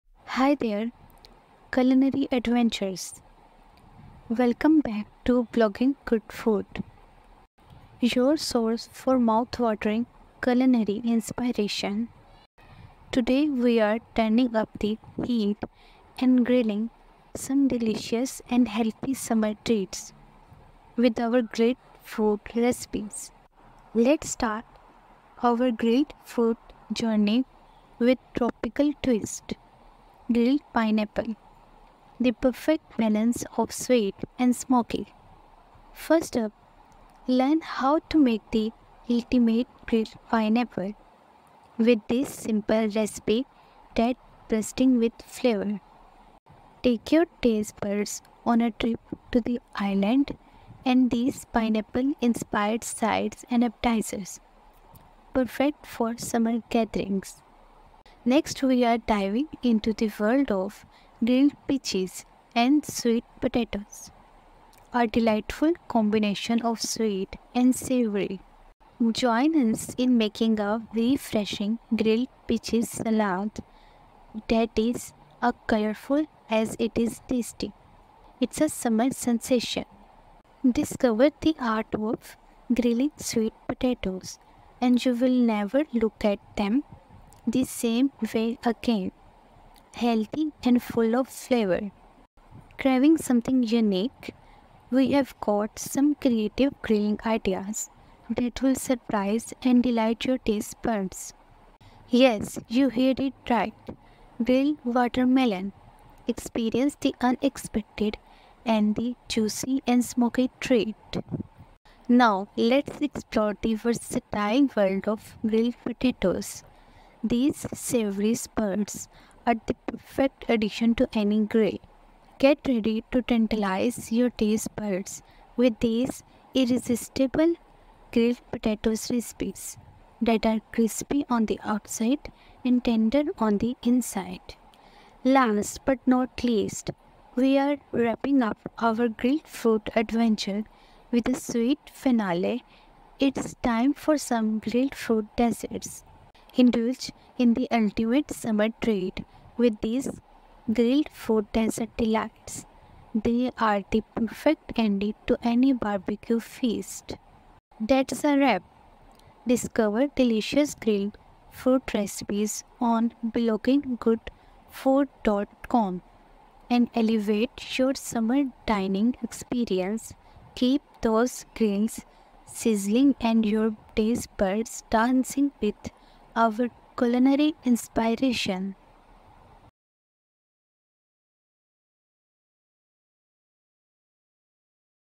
Grilled Fruit Recipes - Delicious and Healthy Summer Treats
Are you looking for a refreshing and healthy way to enjoy your favorite fruits this summer? Look no further than grilled fruit! Grilling fruit brings out its natural sweetness and adds a smoky flavor that is simply irresistible. Plus, it's a great way to add a touch of elegance to your summer gatherings.

In this video, we'll show you how to make a variety of delicious and easy grilled fruit recipes. We'll cover everything from classic pineapple skewers to more adventurous creations like grilled peaches with brown sugar and cinnamon. So fire up your grill and get ready to enjoy some seriously delicious summer treats!

ABOUT OUR CHANNEL:
Our channel is about Food Blogging. We cover lots of cool stuff such as reheating, freezing, defrosting, and cookbooks.